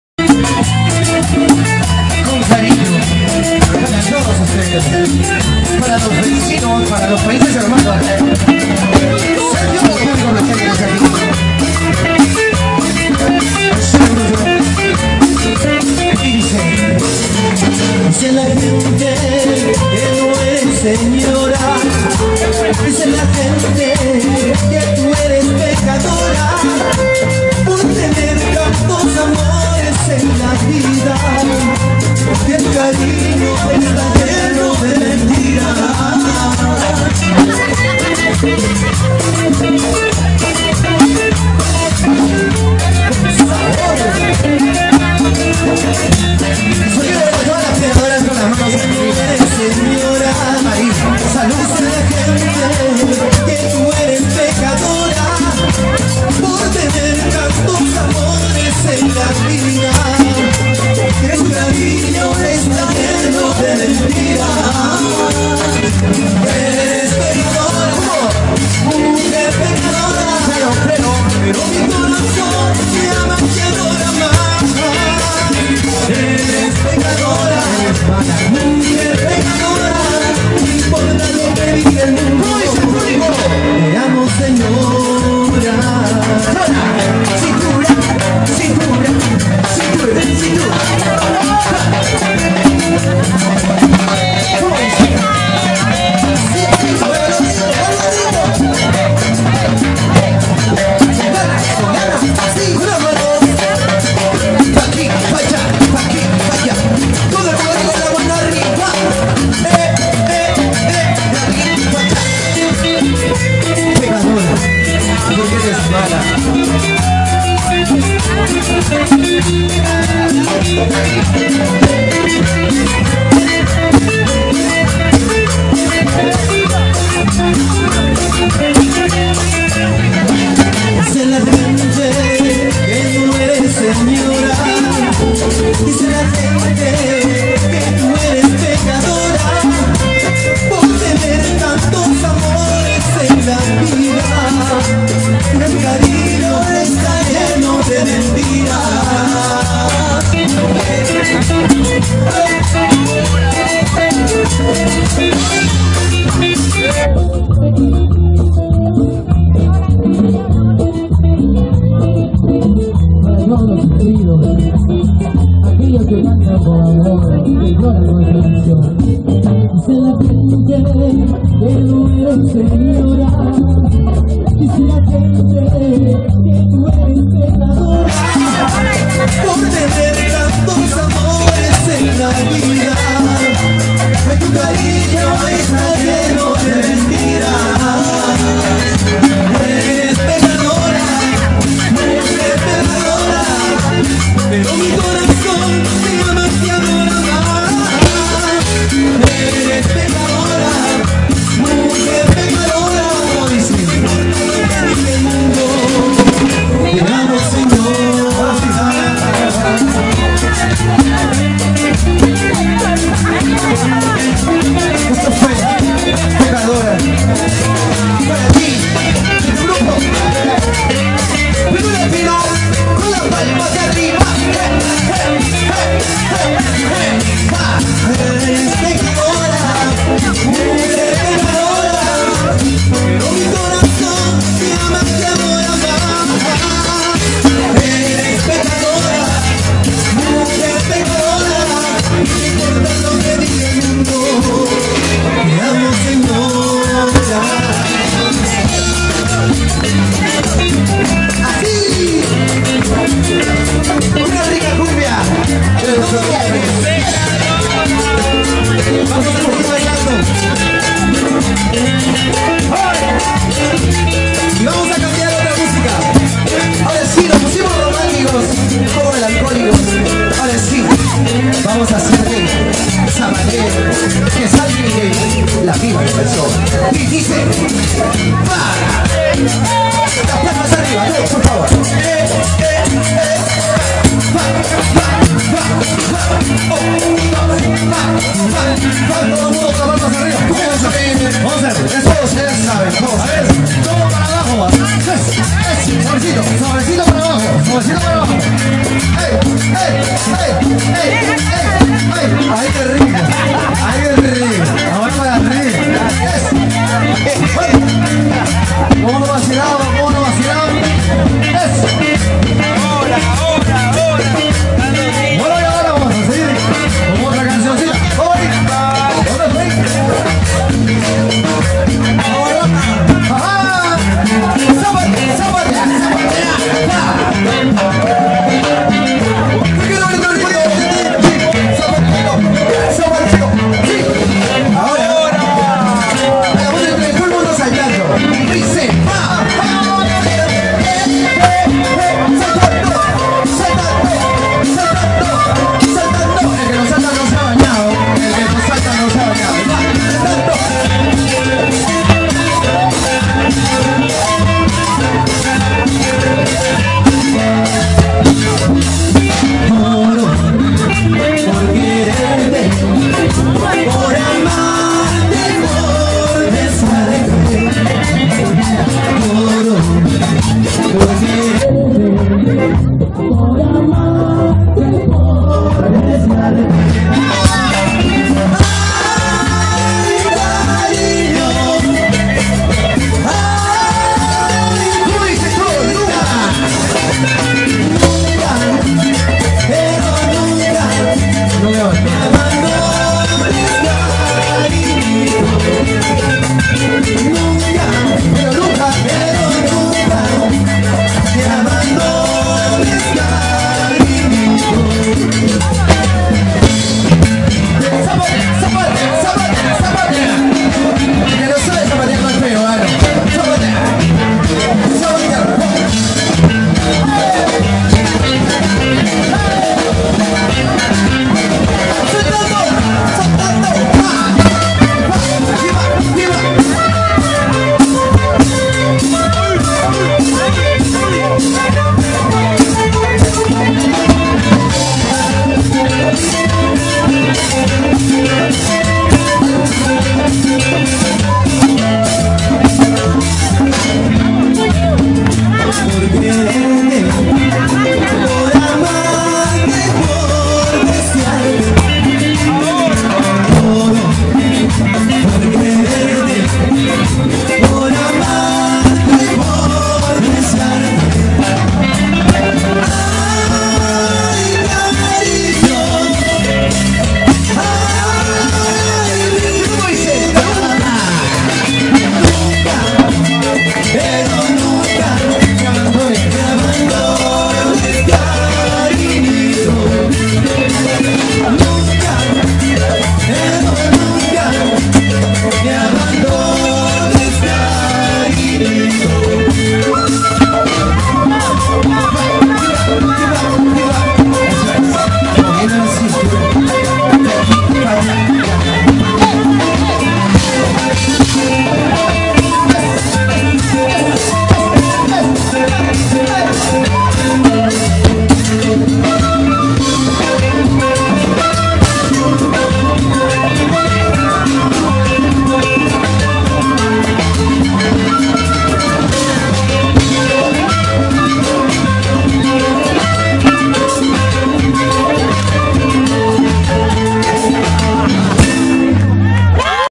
Peru Latino 2013 - Mix Cumbias
Hamburgo - Alemania
Fiesta Stadtpark Hamburg
Fiesta Peruana en Hamburgo